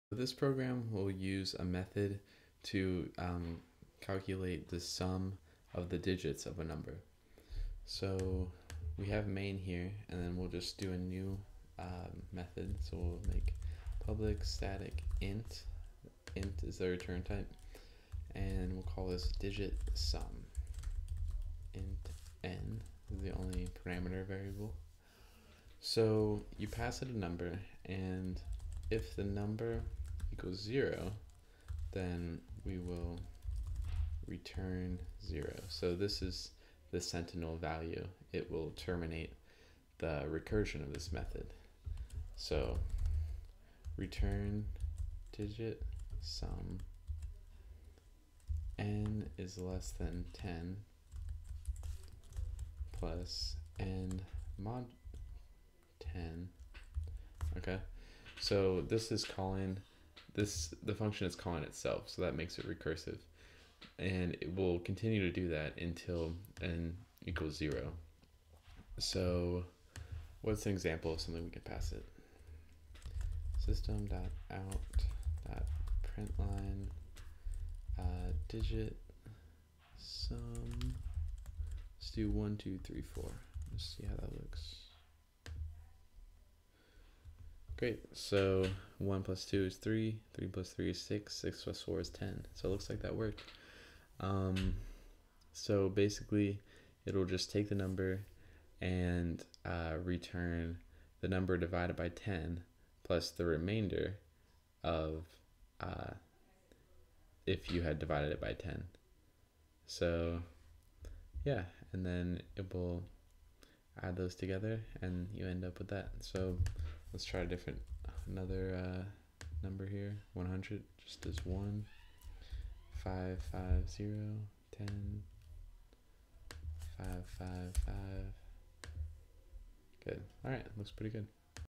Calculate the Sum of Digits in a Number (Java)
A method that calculates the sum of digits in a number.